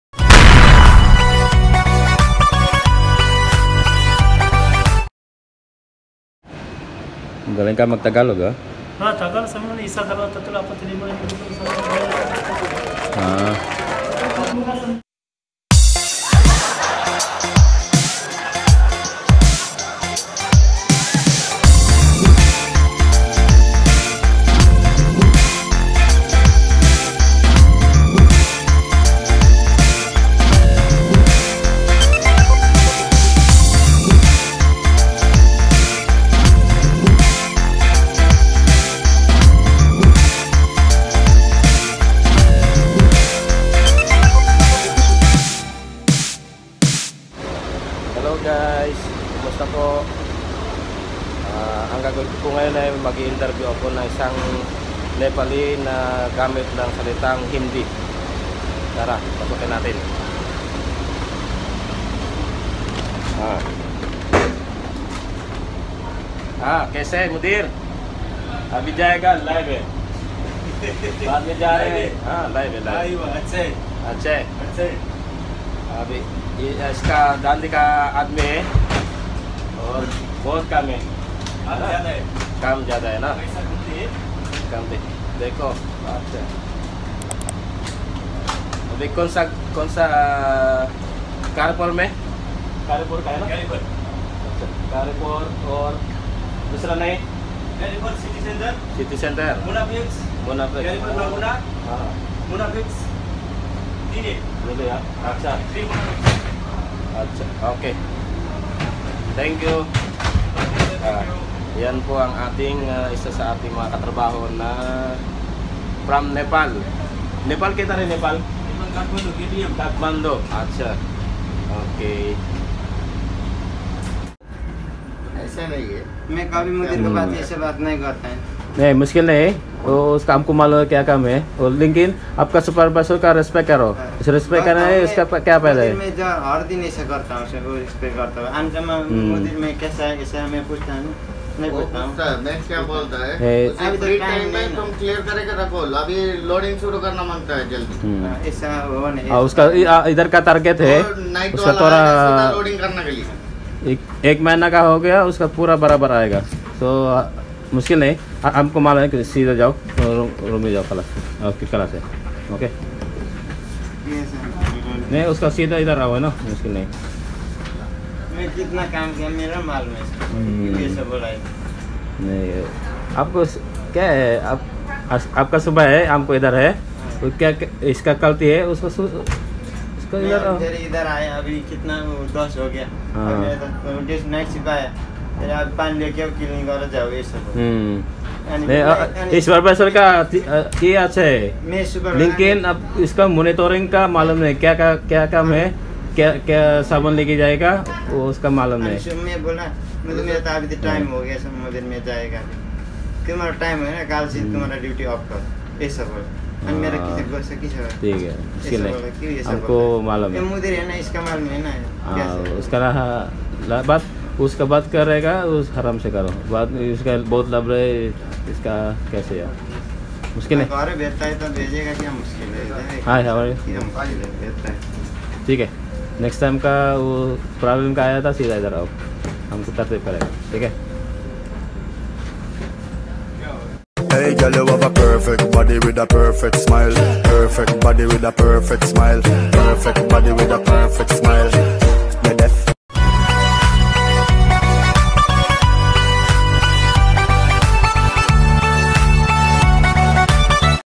Indian guy speaking Tagalog /Arabic Man Eating Ice Cream /Filipino Speaking “HINDI” language
Indian conversion in tagalog
Filipino Well speak HINDI laguage - Indian and Pakistani Urdu language
How Arabic eat Ice Cream cone
Urdu Language easy to learn